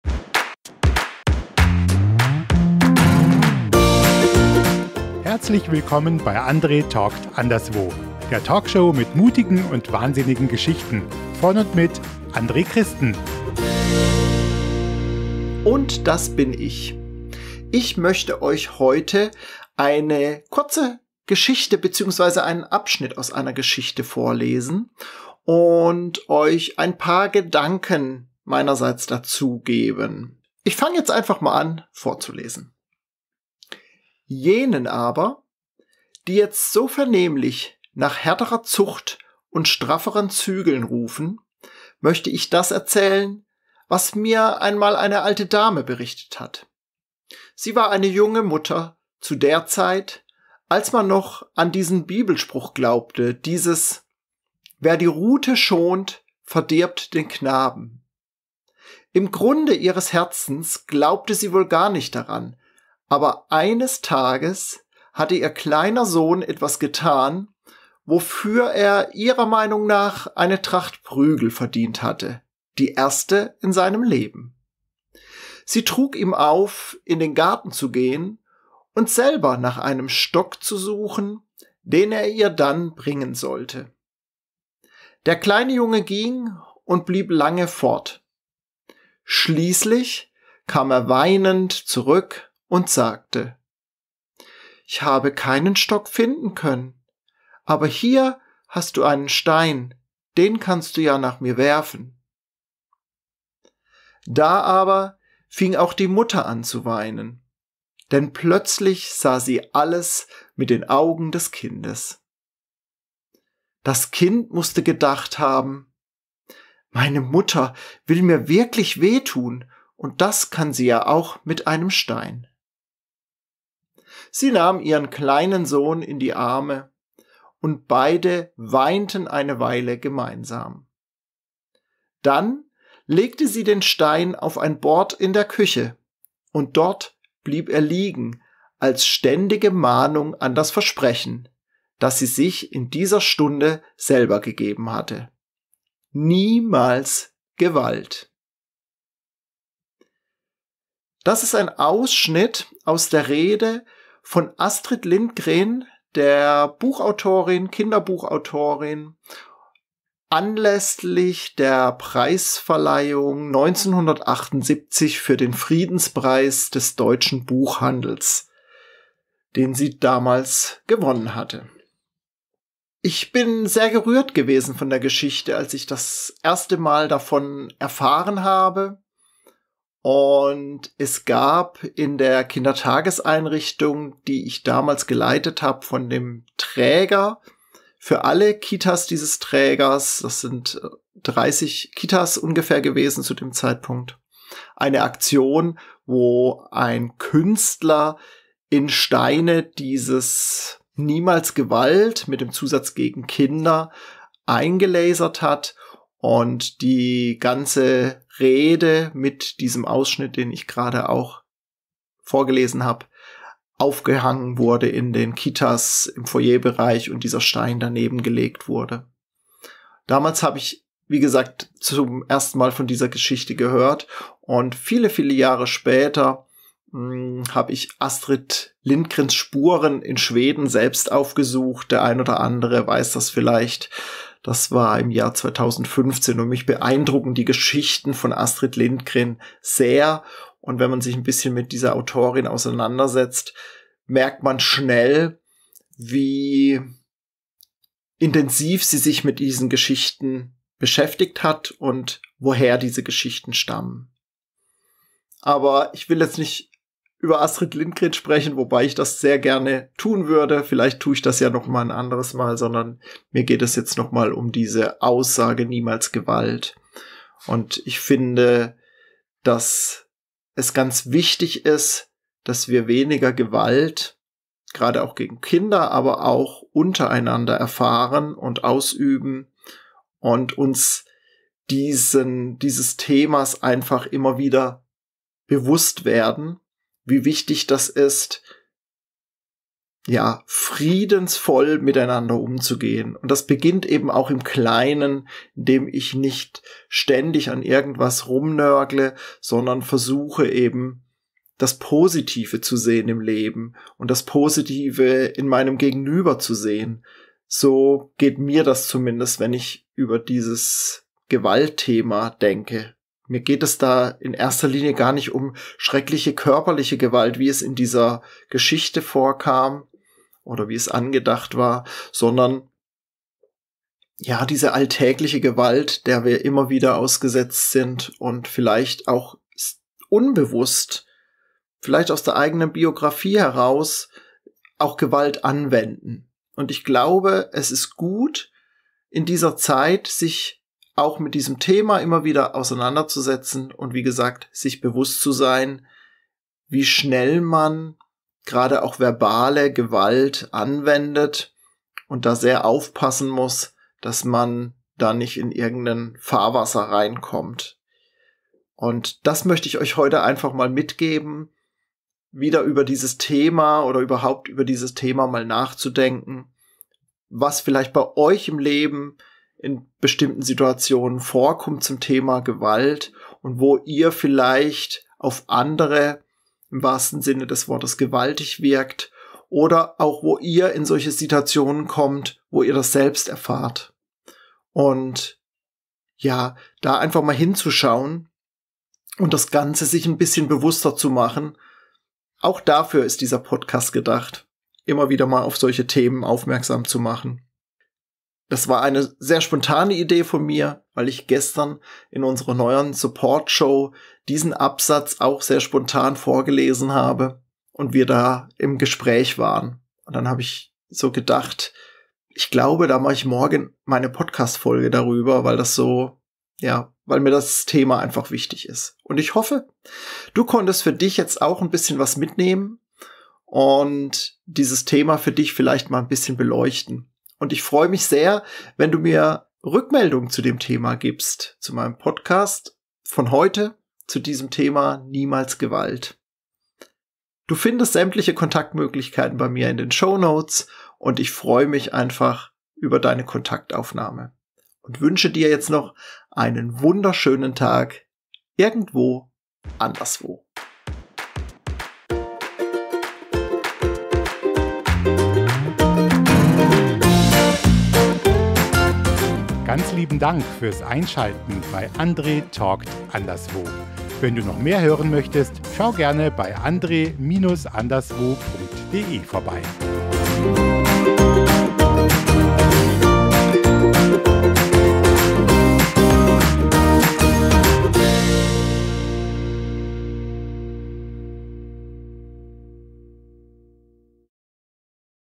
Niemals Gewalt | Astrid Lindgren
Heute möchte einen Auschnitt aus einer kleinen Geschichte vorlesen und dir ein paar Gedanken zum Thema "Niemals Gewalt" mit auf den Weg geben.

 Immer wieder beschäftigen mich auch die verschiedensten Themen im Podcast und ich möchte mit dir diese Gedanken teilen und dich damit inspirieren.

 Als ich das erste Mal diese Geschichte aus der Rede von Astrid Lindgren anlässlich der Verleihung des Friedenspreises des Deutschen Buchhandels (1978) gehört hatte, war ich sehr berührt.

 Ich erzähle im Podcast, warum ich diese Geschichte hörte und welche Gefühle bzw. Überlegungen ich dazu habe.

 Über deine Rückmeldung freue ich mich besonders.

Rede von Astrid Lindgren anlässlich der Verleihung des Friedenspreises des Deutschen Buchhandels 1979